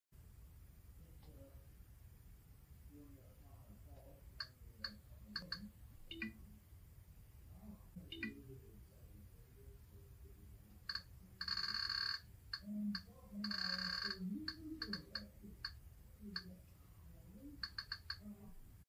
Mote: Control Roku TVs From Your Terminal!
Quick demo of a project I just released: Mote, a command line applications for discovering and controlling Roku TVs on your network.

I'm constantly losing my remote, so I would prefer to just open a shell to control my TV.